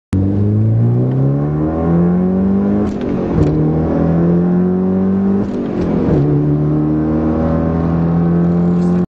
2a-3a-4a marcia cambiata a gas aperto su lancer evo
rimappatura centralina con cambiata a gas aperto (tutto accelerato mentre c'è la frizione giù) su mitsubishi lancer evolution
mantiene il turbo in pressione mentre si cambia marcia, senza stressare la meccanica (frizione- trasmissione)
in questo video potete vedere un passaggio dalla 2a alla 3a e 4a tutto senza lasciare l'acceleratore.
disponibile anche per subaru